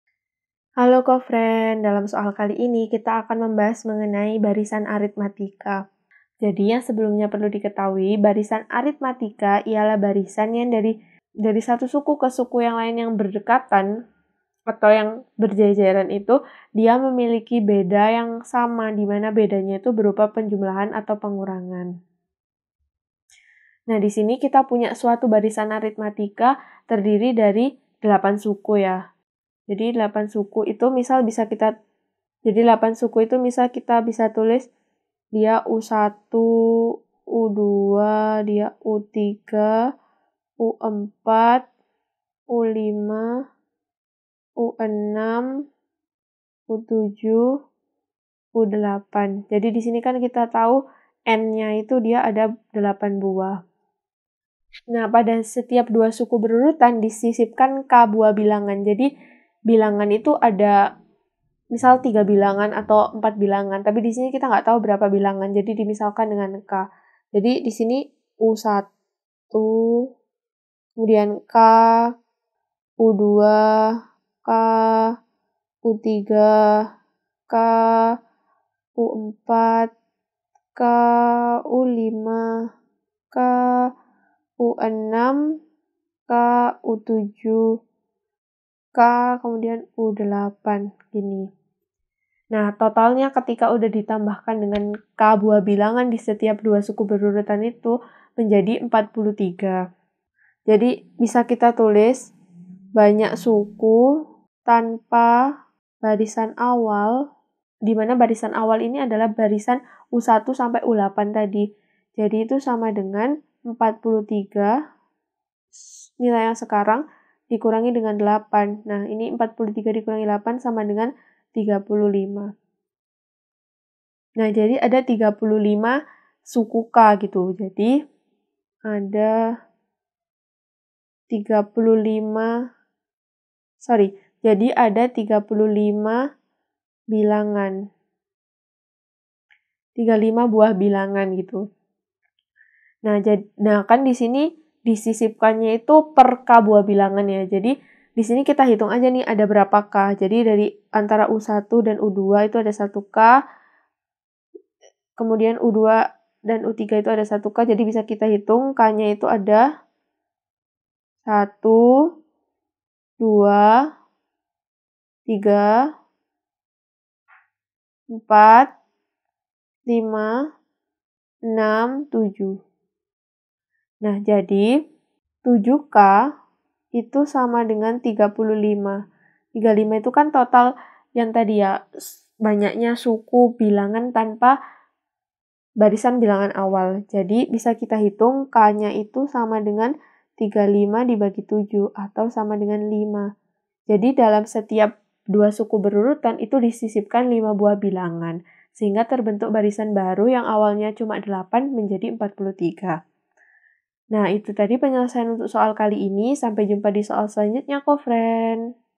Suatu barisan aritmatika terdiri dari 8 suku. Pada setiap dua suku berurutan disisipkan k buah bi...
Suatu barisan aritmatika terdiri dari 8 suku. Pada setiap dua suku berurutan disisipkan k buah bilangan sehingga terbentuk barisan baru. Jika barisan yang baru  terdiri dari 43 suku, maka nilai k adalah

Topik atau Materi: Barisan Aritmetika - Barisan - ALJABAR - Matematika Kelas 11 SMA -

Sumber Soal: Matematika Kemendikbud Rumah Belajar SMA , Kemendikbud

00:00 : Bedah Soal
01:00 : Konsep, rumus dan pengertian Barisan Aritmetika
01:18 : Langkah penyelesaian soal
02:28 : Jawaban akhir
02:58 : Penutup